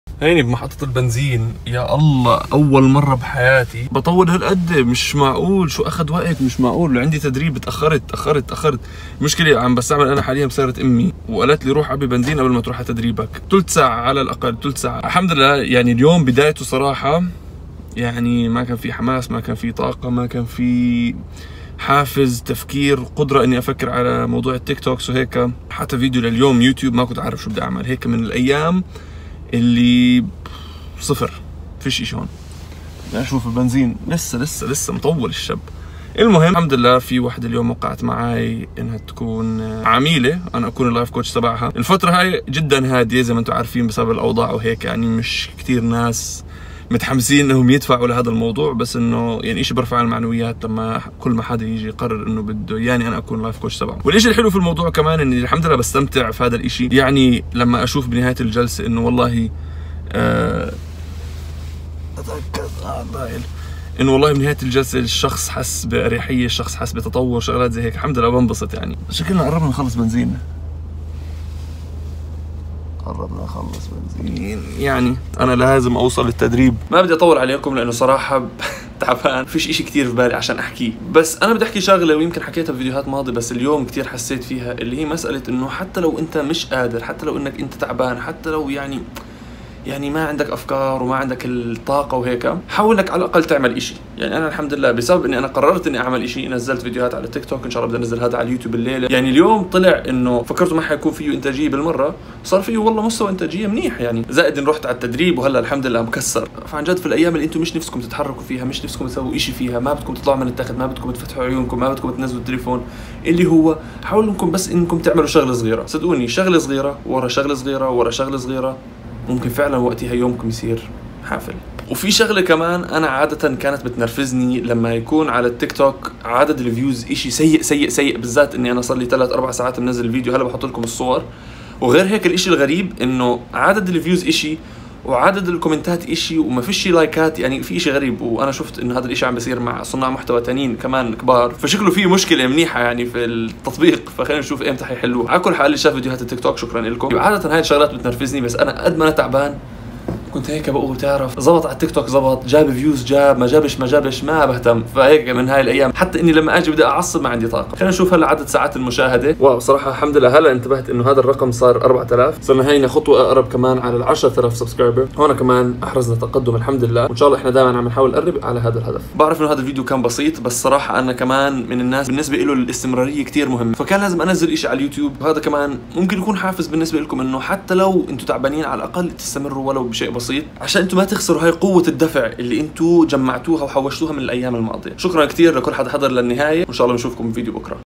ما في داعي تشوف هاد الفيدو
فيديو بسيط هدفه الاستمرارية. رغم التعب اللي حاسس فيه كان لازم أنزل فيديو عشان أحافظ على وعدي أني إن شاء الله راح أنزل فيديوهات يوميه على اليوتوب